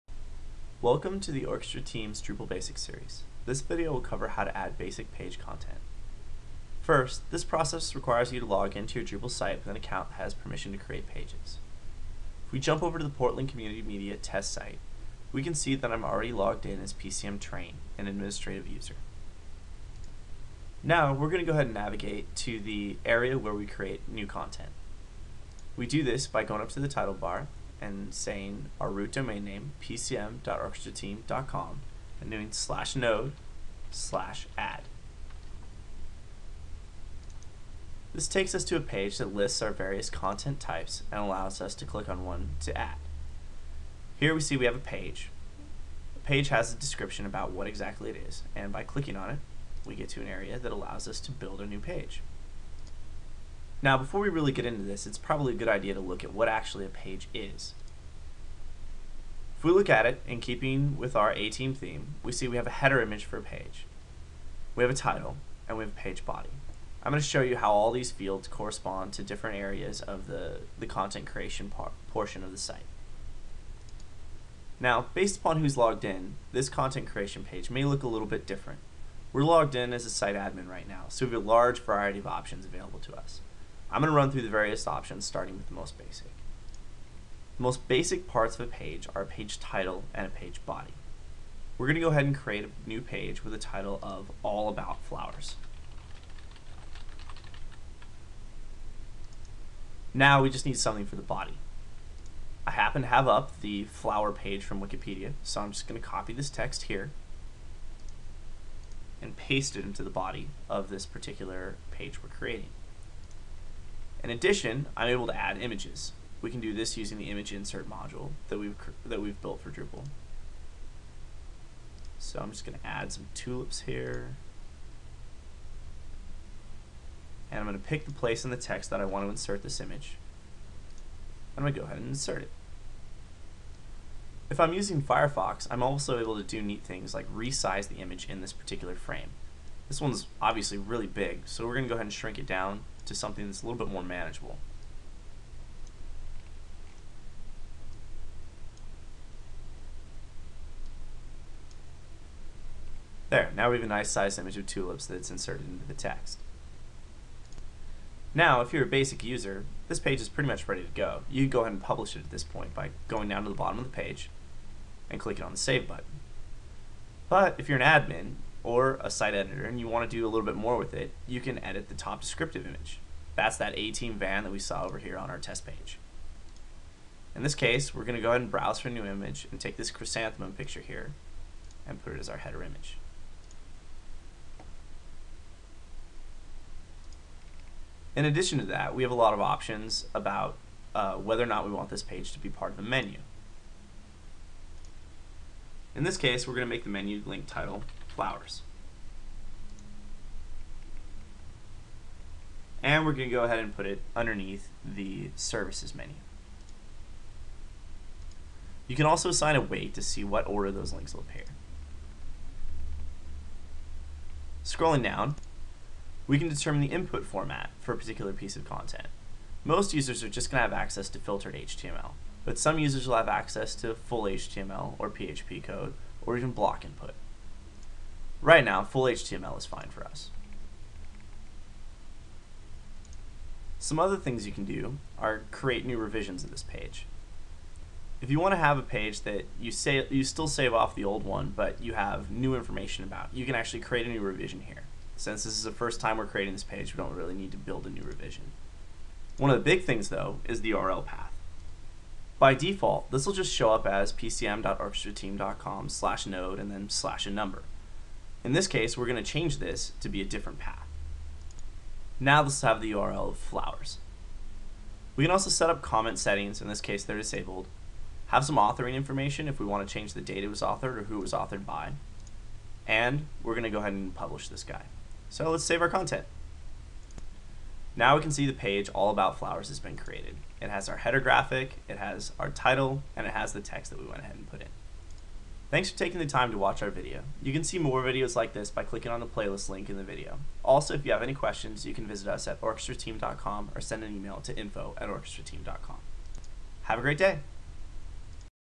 Drupal Basics - Creating Page Content
This shows how to create pages in Drupal. The pages include some basic CCK fields, the TinyMCE rich text editor, and the image insert module. It also has some basics about page revisions, menus, and additional features.

The sample site is for a client - Portland Community Media